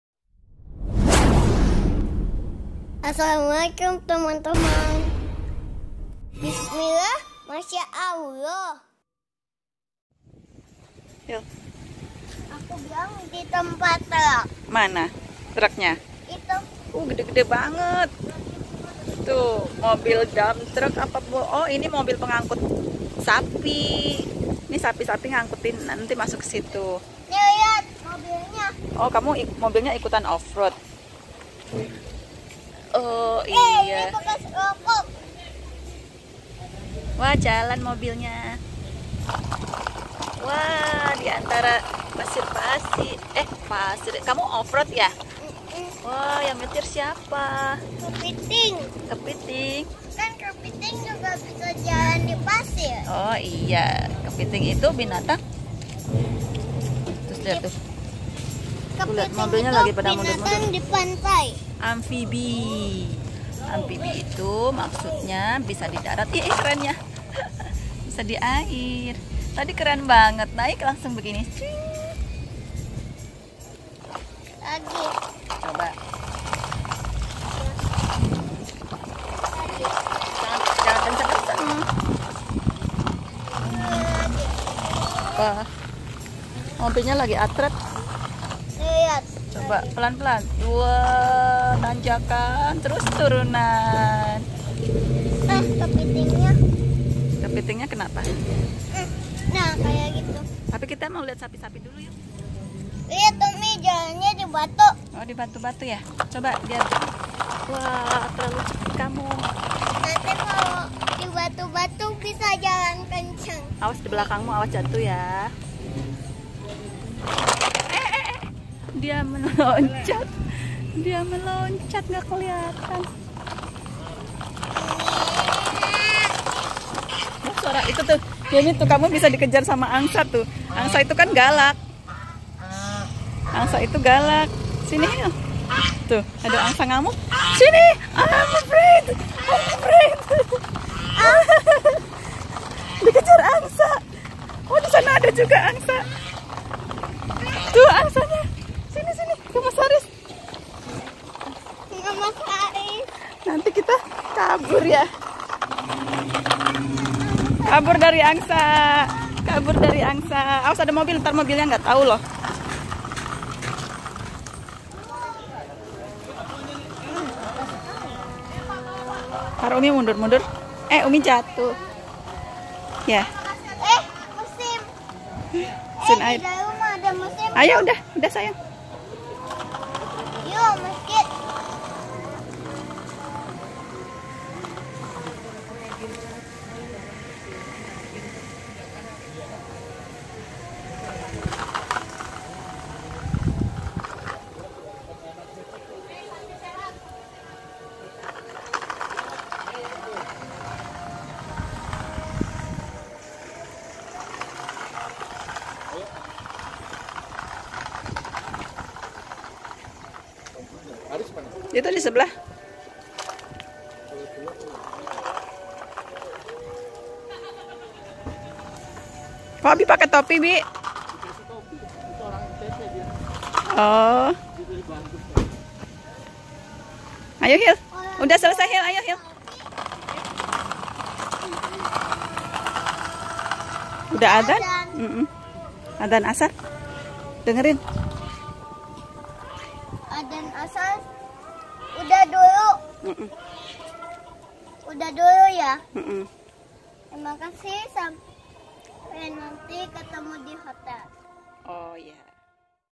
Dikejar2 Angsa saat main off road. Takuuuut...
Pas kita lagi main2 mobilan off-road di tempat truck2,  eh ternyata ada angsa2 , trus dikejar2.. takut dah, ayo lariii